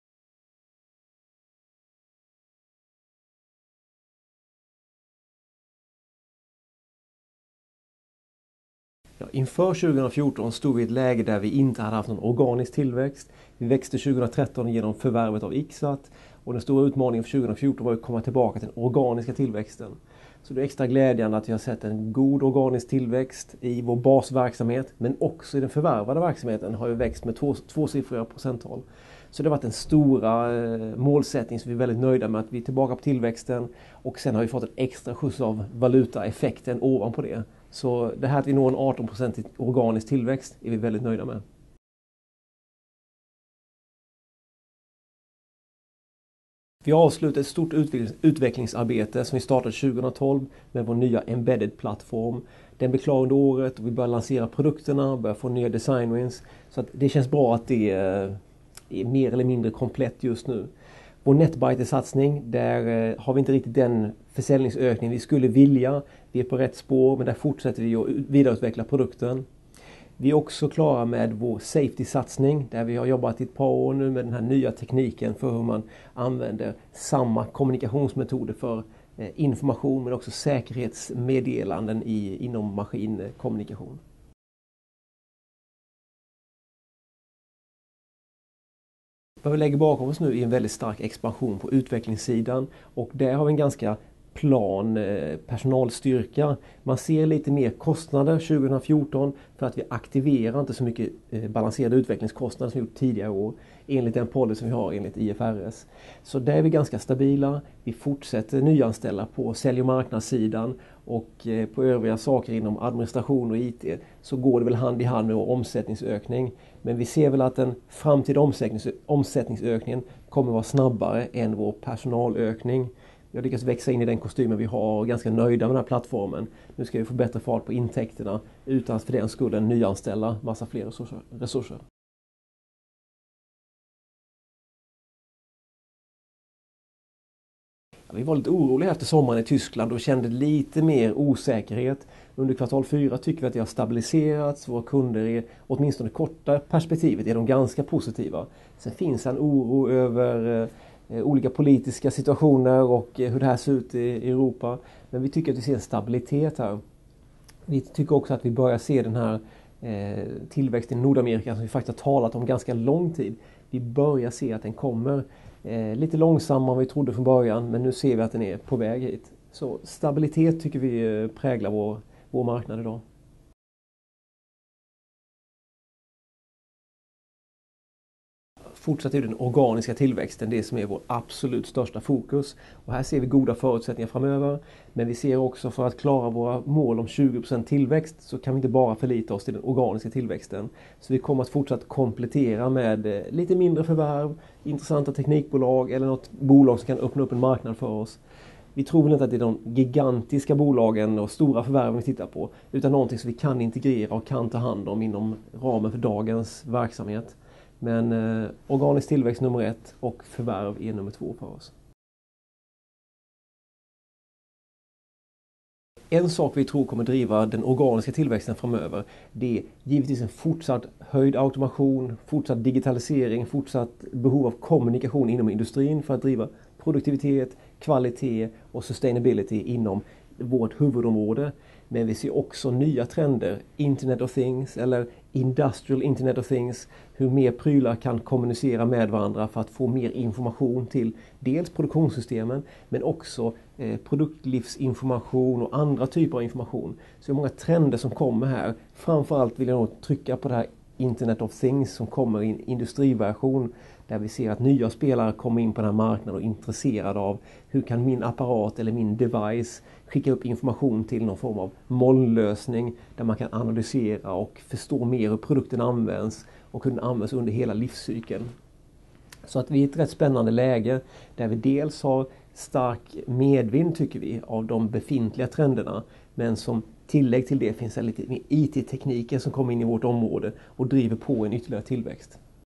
HMS Networks - Intervju med VD Staffan Dahlström
Inspelad 12 februari 2015

HMS Networks VD Staffan Dahlström sammanfattar det föregående verksamhetsåret och jämför med planen det kommande året. Vidare kommenterar han vilka produkter som kommer vara viktigaste att få igång 2015 och hur mycket ytterligare HMS Networks kan växa utan att utöka personalstyrkan. Han fortsätter med att beskriva situationerna i Tyskland och USA, samt hur de ställer sig till organisk respektive förvärvad tillväxt framgent, nu när IXXAT är integrerat. Staffan avslutar intervjun med att förklara vilka trender det är som i framtiden kommer driva den organiska tillväxten.